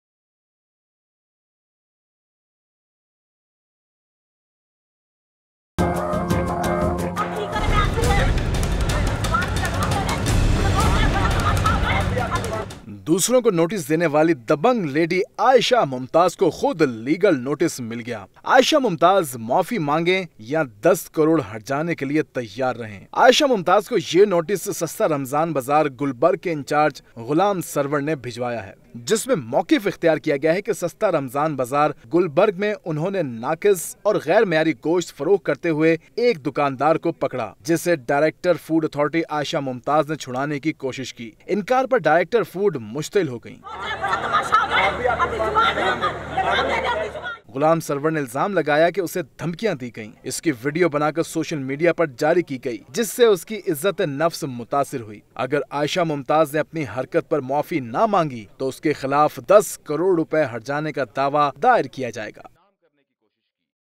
24 Report: Ramadan bazaar incharge sends legal notice to Ayesha Mumtaz
Ramadan bazaar incharge sends legal notice to Ayesha Mumtaz